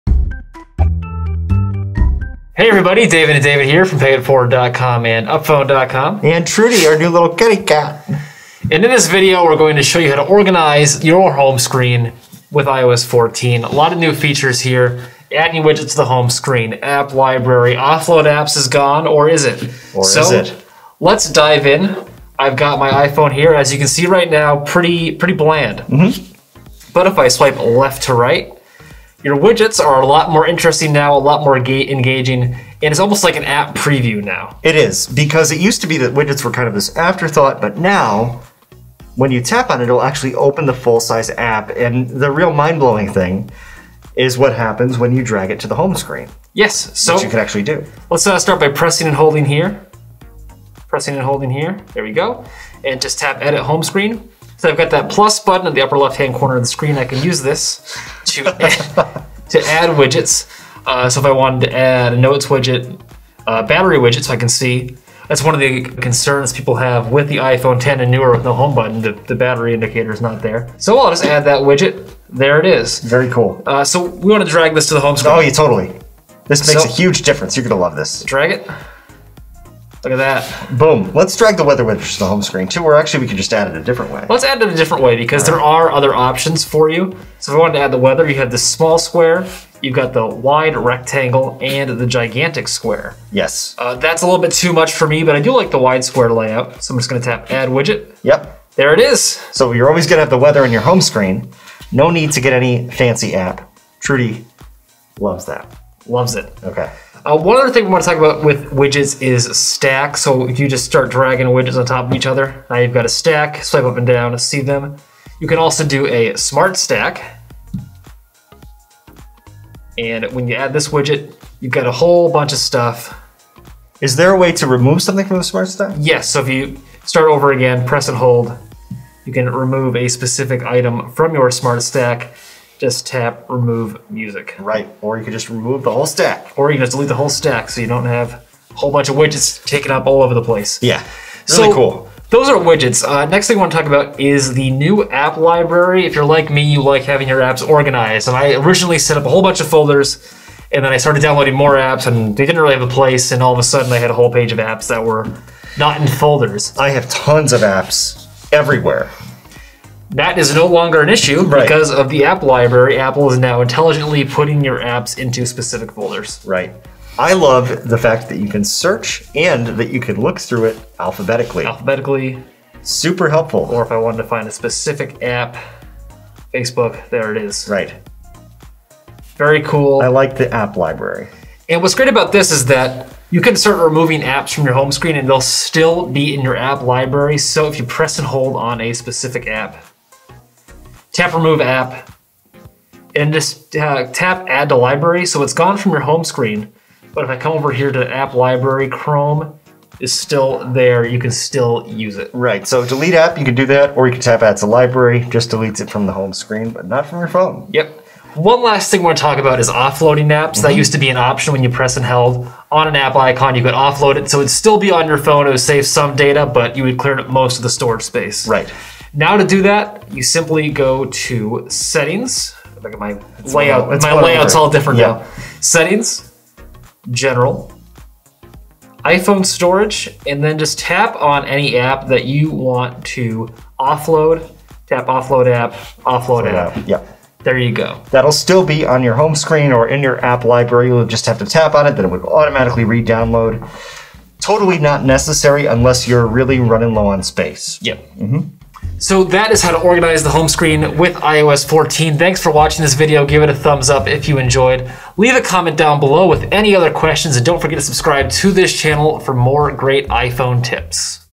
How To Customize Your iPhone Home Screen [iOS 14]
Apple experts show you how to customize the iPhone Home Screen with iOS 14. You can now add a variety of widgets to the Home screen and keep your apps organized with the App Library.